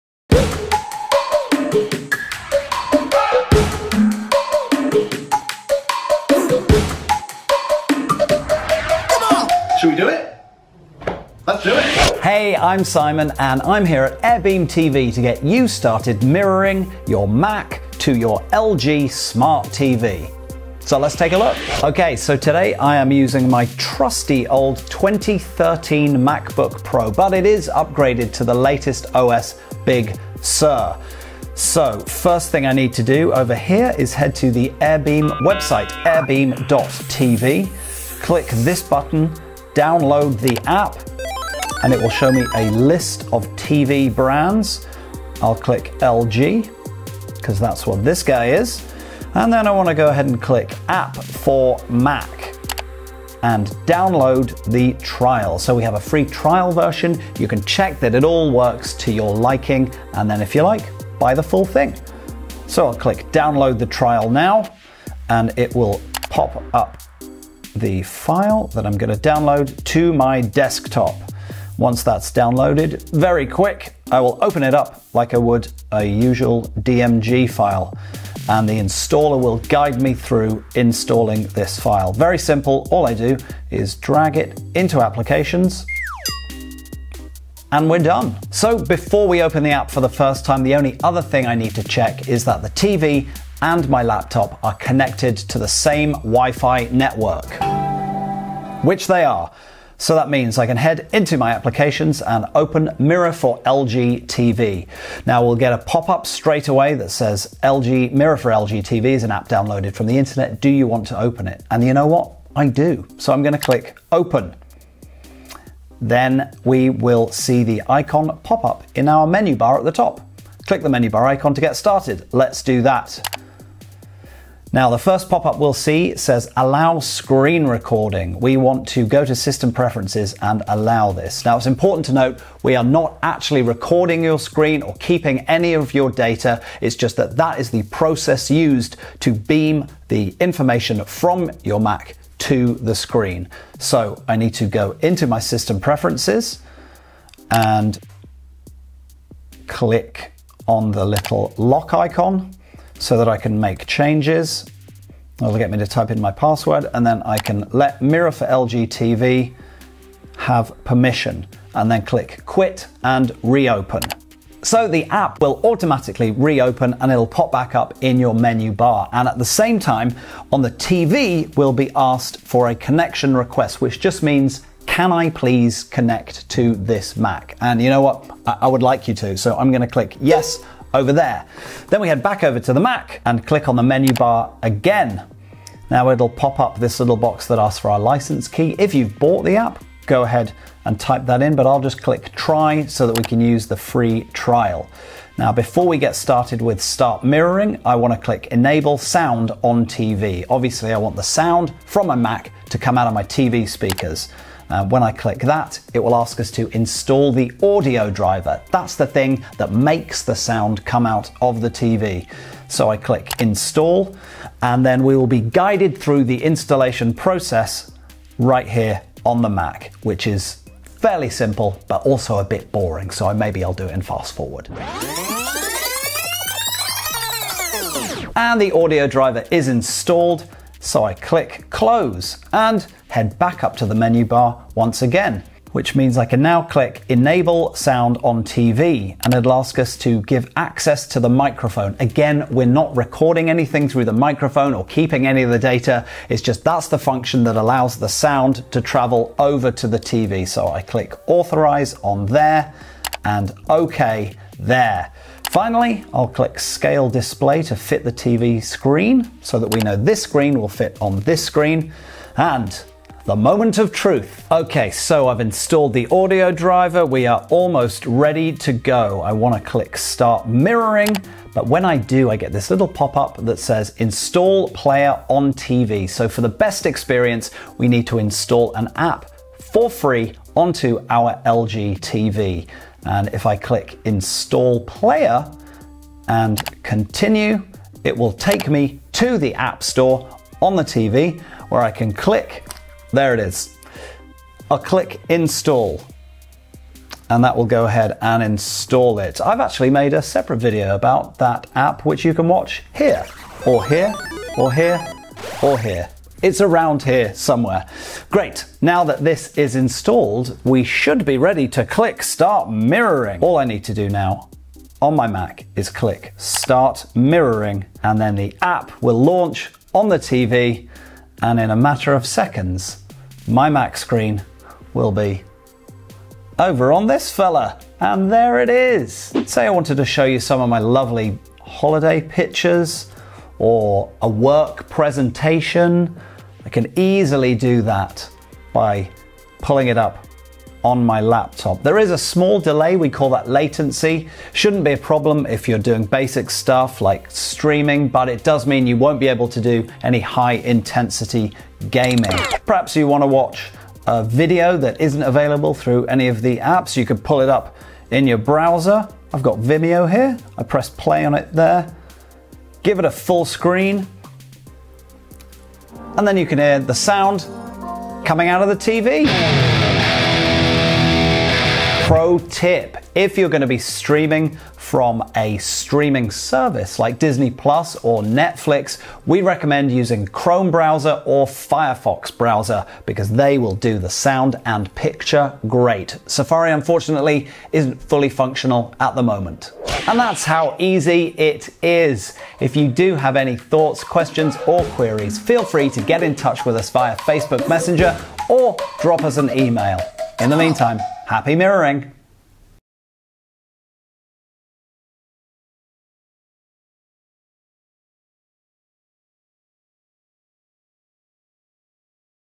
App: Connect MacBook To LG Smart TV (WIRELESSLY) Without AirPlay or Apple TV | 2025 | AirBeamTV
In this video you learn how to connect a MacBook to your LG TV. This screen mirroring app from AirBeamTV uses the latest technology just to do that!

You can cast your Mac, MacBook, iMac or Mac Mini display without the need for an expensive Apple TV or HDMI-cord. Watch this short video from Simon to find out how and you will be up and running in no time!

Tips and tricks:
* Use Chrome or Firefox browser for the best streaming performance for services like Netflix, Disney Plus, Prime Video or Hulu.
* Our app will work with all LG Smart TVs from 2012 and newer
* Install the free Receiver App from the LG App Store on your TV (if your TV model supports this)

Stream movies, share photos or cast videos straight from your Mac to your LG TV for free! Happy after the trial? Buy the full version for just $9.99. The Premium version includes lifetime updates and unlimited and ad free screen mirroring.

Happy Mirroring!